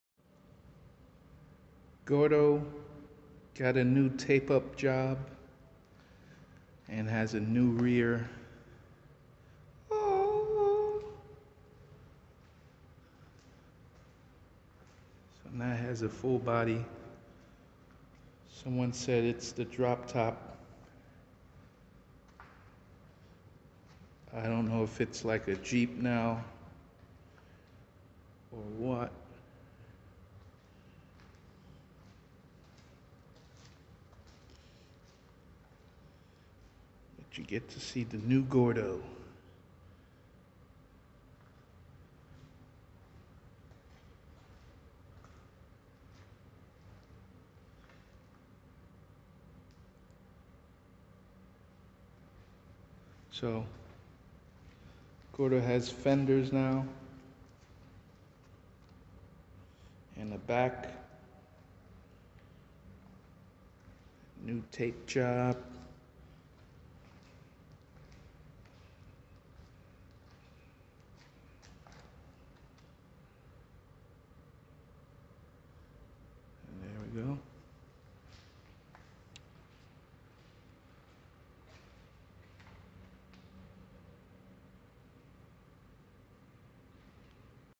Gordo is officially a velocar!
I added fenders and a back frame to make Gordo look more like a velocar. What's your opinion on the look?